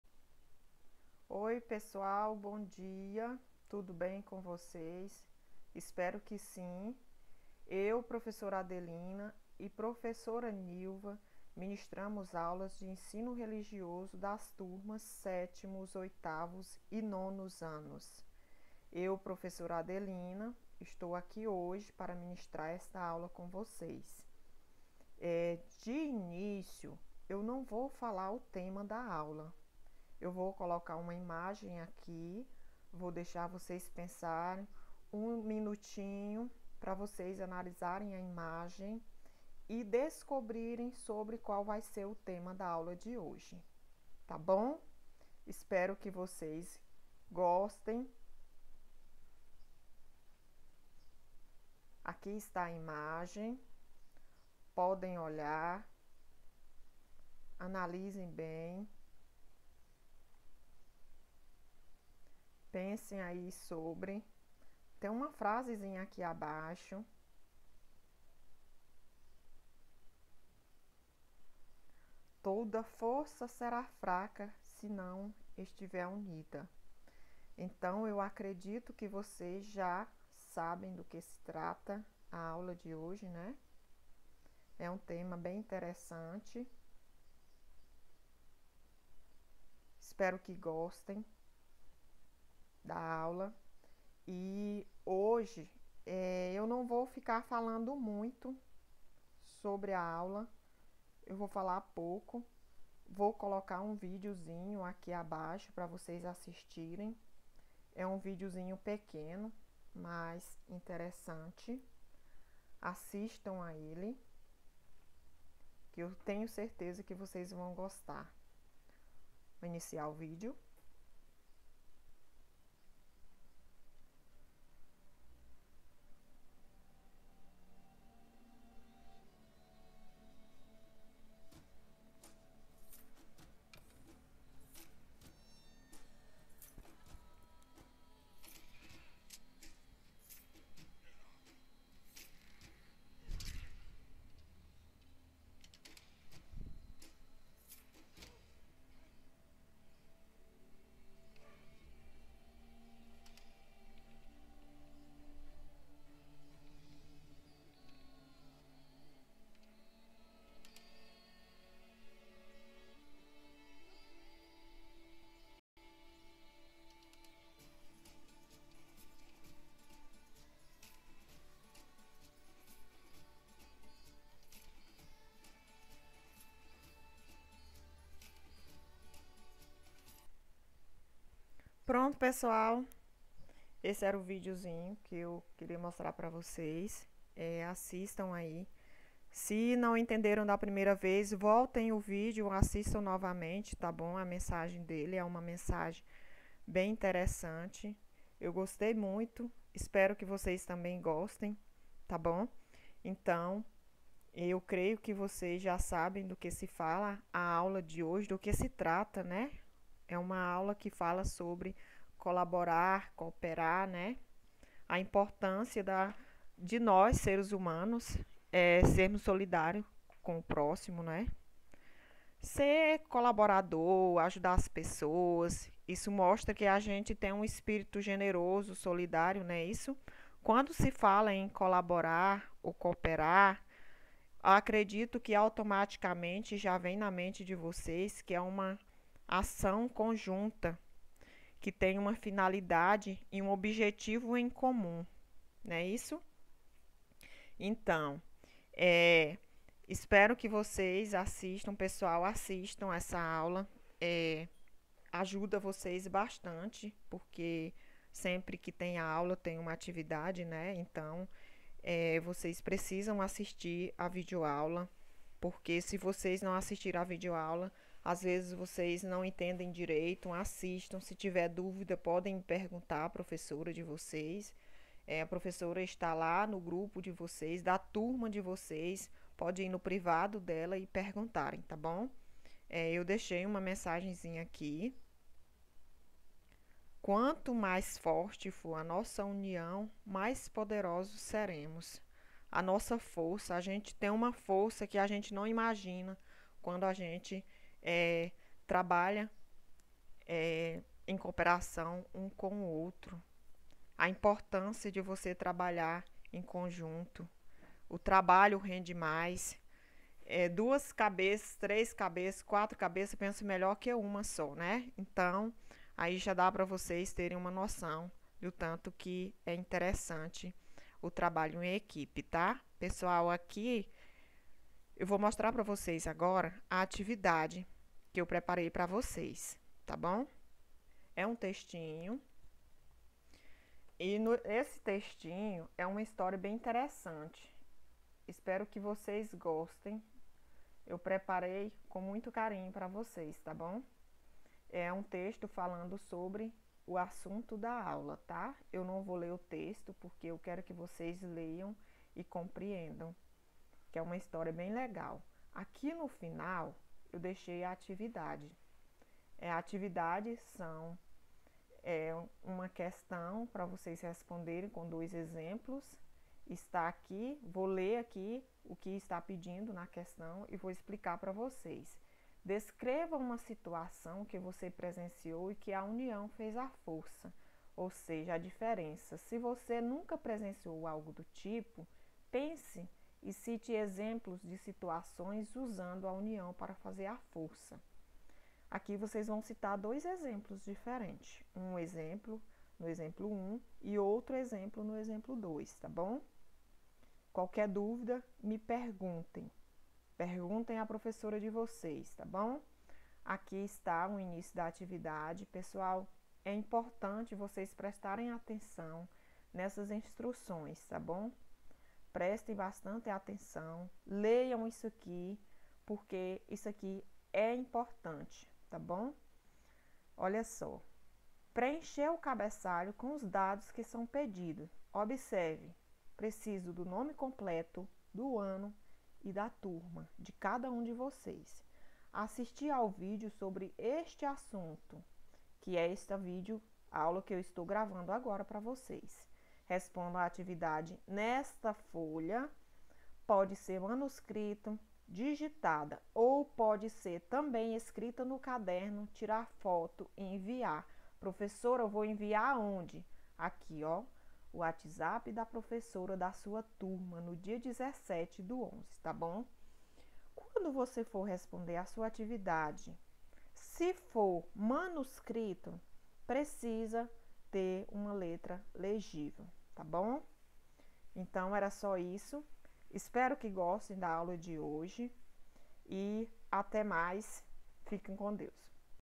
Aula 3 - Ensino Religioso. Tema: Cooperação. Prof.ª Adelina - para alunos dos 7°s, 8°s e 9°s Anos.
Aula remota desenvolvida para alunos da Escola Municipal José Cardoso de Lima.

Link da atividade em PDF - AULA 3.